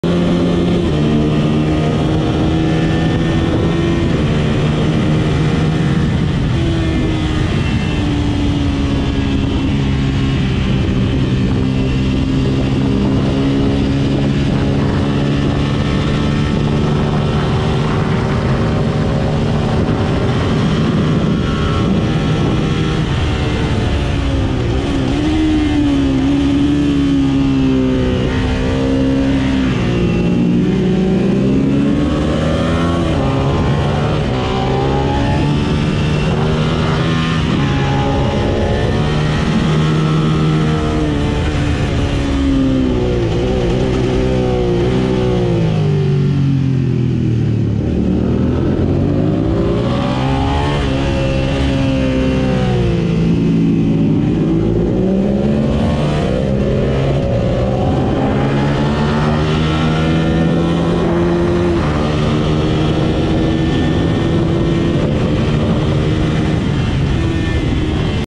Minitwin race 1, Mallory 2Jun24 Phil's fastest lap 1.00.9 EMRA
Minitwin race 1, best of day, and ever. Gen1 Suzuki SV650